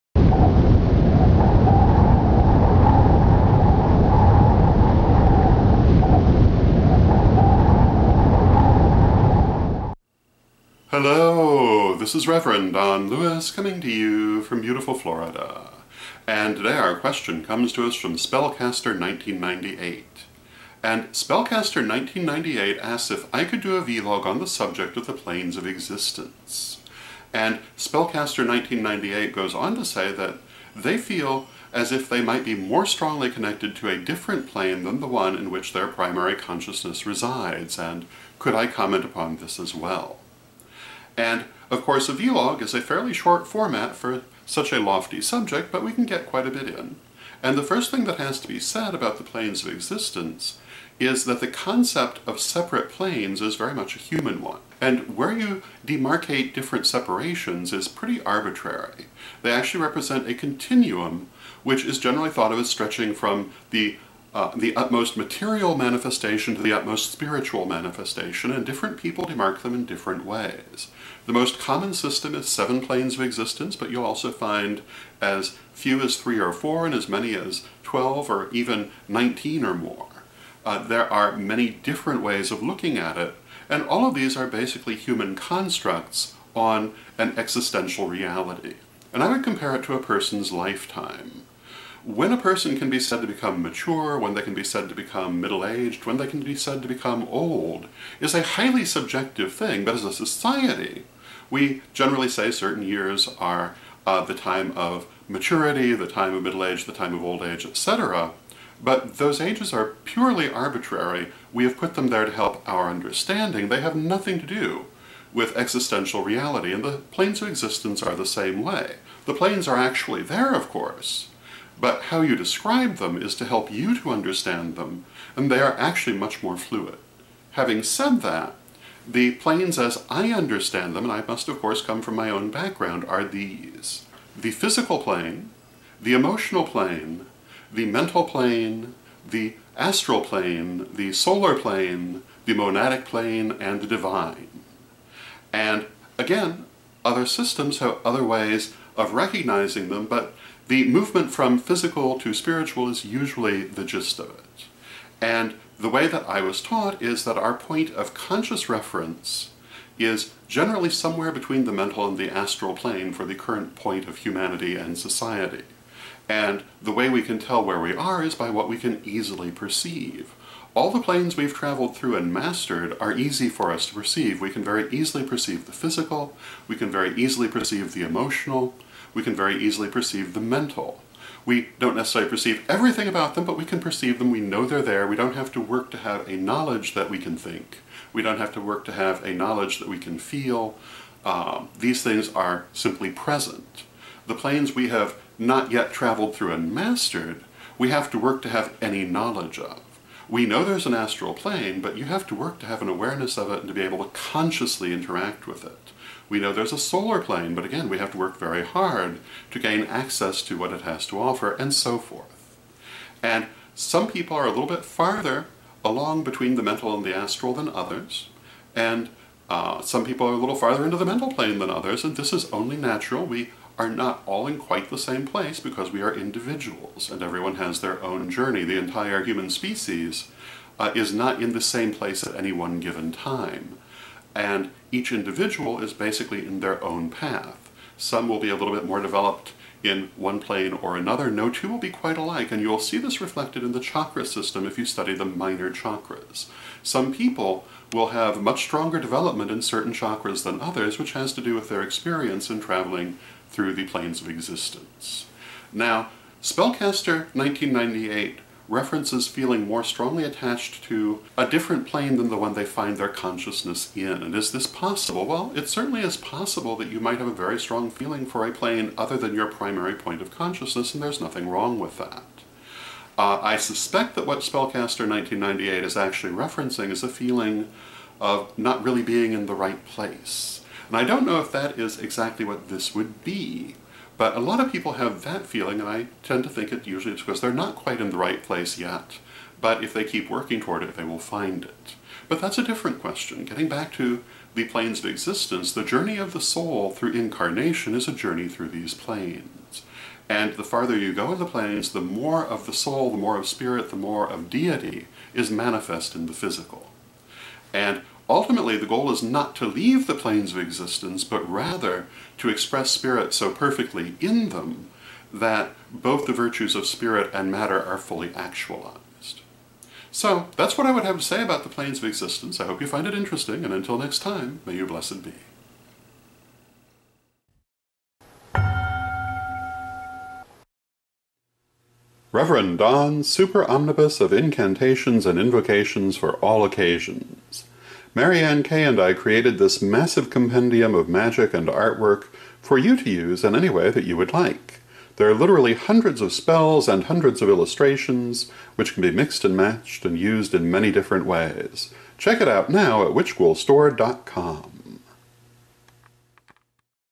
Rev. Don's Vlog - Planes of Existence
Rev. Don discusses the Planes of Existence, in response to a question from Spellcaster1998. Join Rev. Don Lewis for his weekly Chancellor's Chat, tonight at 8 PM Central, 9 PM Eastern! You never know what we might end up talking about -magic and ritual, theology, history, art, social issues, etc but it's always interesting.